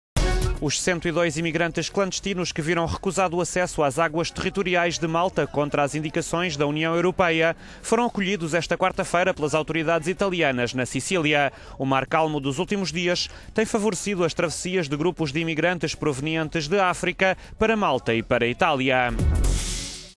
Grupo de clandestinos recusado por Malta chega à Sicília
Os 102 imigrantes clandestinos que viram recusado o acesso às águas territoriais de Malta, contra...

Os 102 imigrantes clandestinos que viram recusado o acesso às águas territoriais de Malta, contra as indicações da União Europeia, foram acolhidos esta quarta-feira pelas autoridades italianas, na Sicília. O mar calmo dos últimos dias têm favorecido as traversias de grupo de imigrantes provenientes de África para Malta e para a Itália.